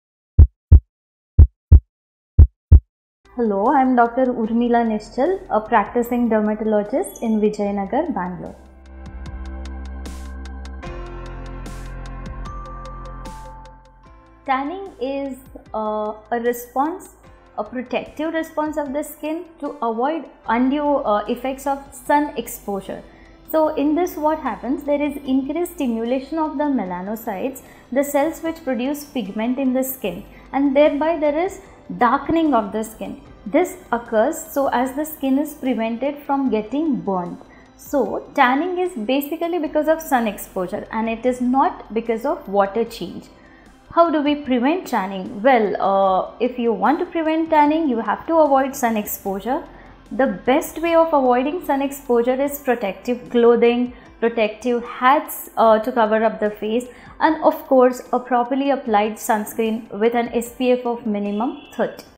Can skin tan happen due to water change? - Dr. Urmila Nischal
Tanning is a protective response of skin to protect from sun exposure. There is increased production of melanocytes which produce pigment in skin which causes darkening of skin. Tanning is due to sun exposure not water change. Protection against sun rays is by full sleeves clothing, sunscreen with UVA protector.